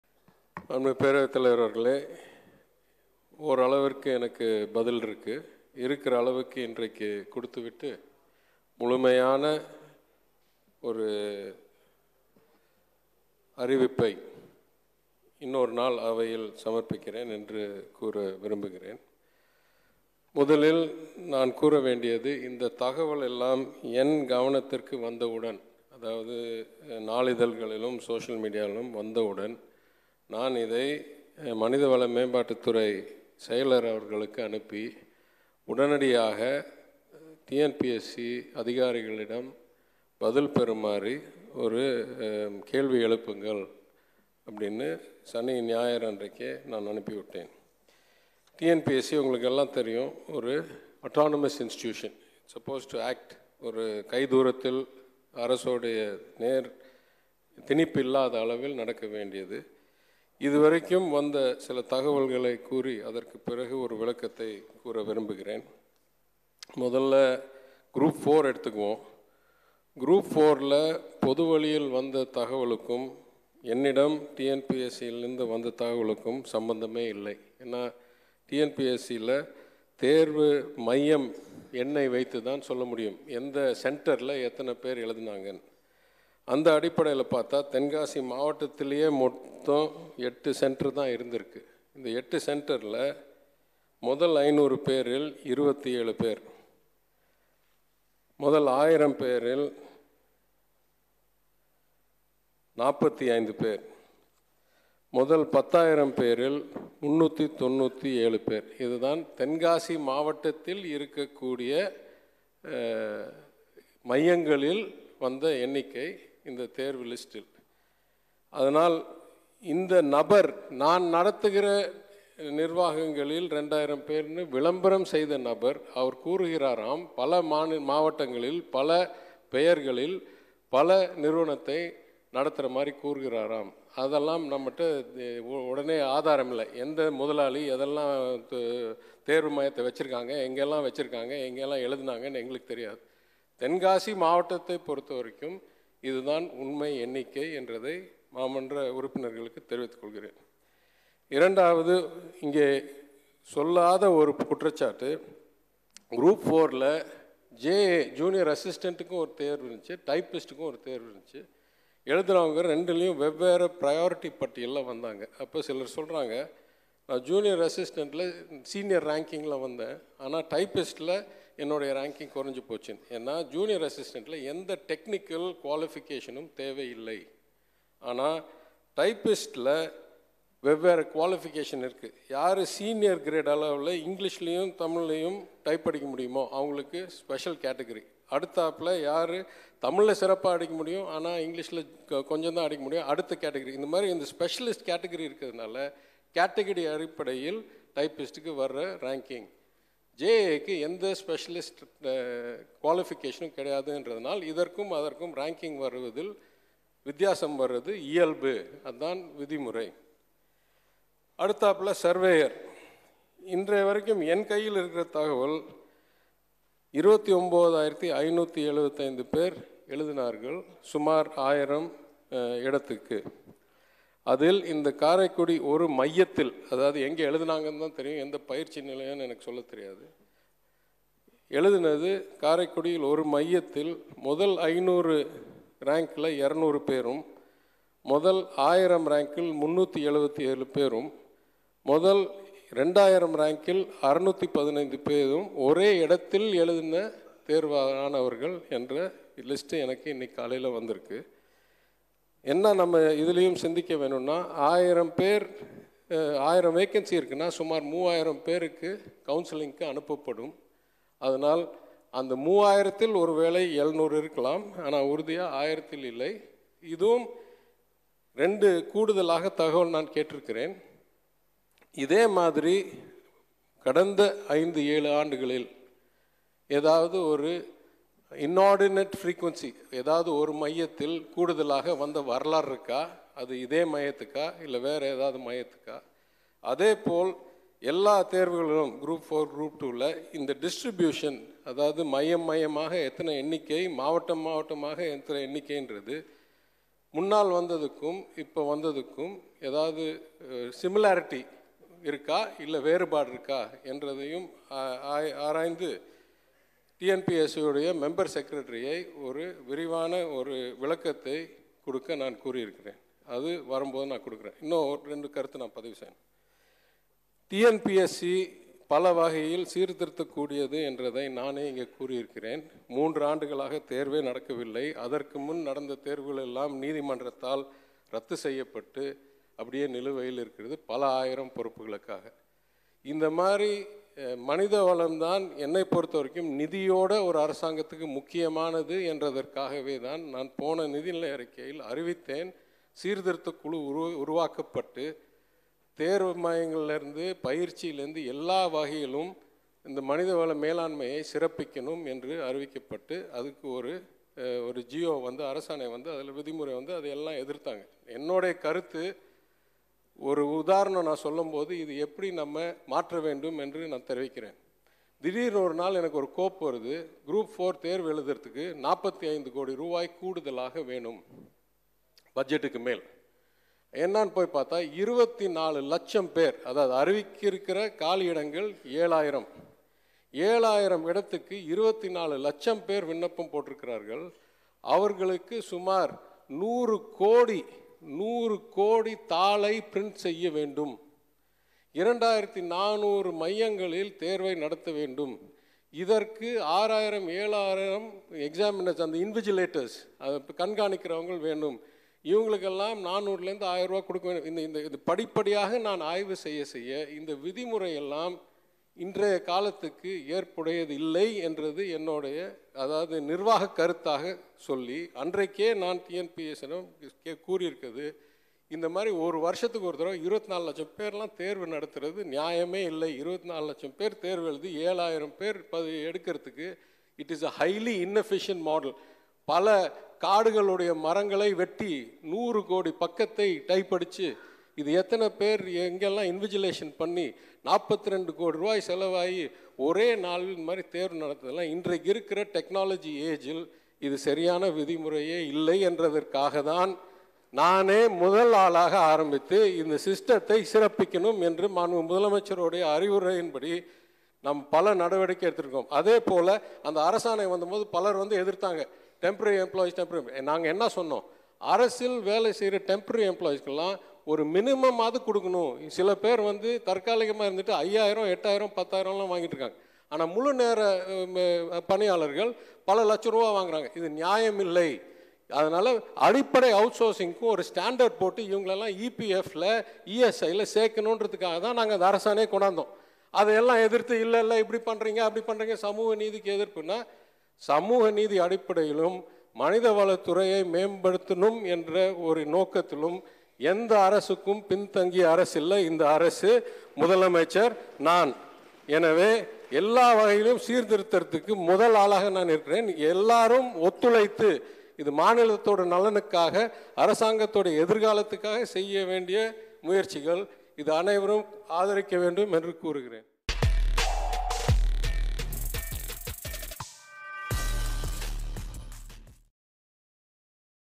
எந்த அரசுக்கும் பின் தங்கிய அரசு இல்ல இந்த அரசு.. | அமைச்சர் PTR அதிரடி! | TN Assembly | TNPSC
எந்த அரசுக்கும் பின் தங்கிய அரசு இல்ல இந்த அரசு.. | அமைச்சர் PTR அதிரடி!